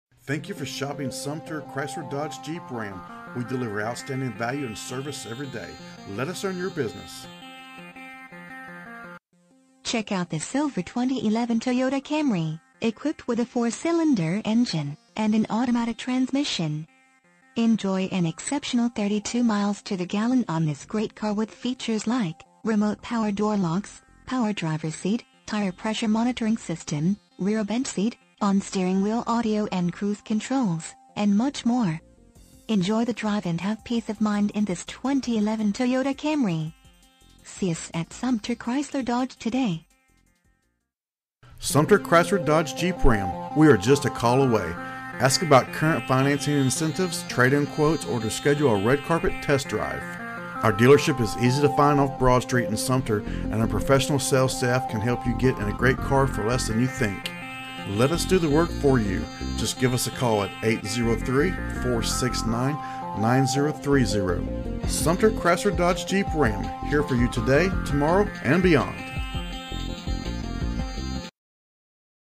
2011 Toyota Camry Sumter Chrysler Dodge Sumter, SC 29150
Toyota Camry Check out this Silver Toyota Camry 4 Dr Sedan, equipped with a 4 Cylinder engine and an automatic transmission. Enjoy an exceptional 32 miles to the gallon on this great car with features like Remote Power Door Locks, Power Driver's Seat, Tire Pressure Monitoring System, Rear Bench Seat, on steering wheel audio and cruise controls, and much more. Enjoy the drive and have peace of mind in this Toyota Camry. See us at Sumter Chrysler Dodge today!
MILES: 24,400
VIN:4T1BF3EK5BU722803

2662 Broad Street
Sumter, South Carolina 29150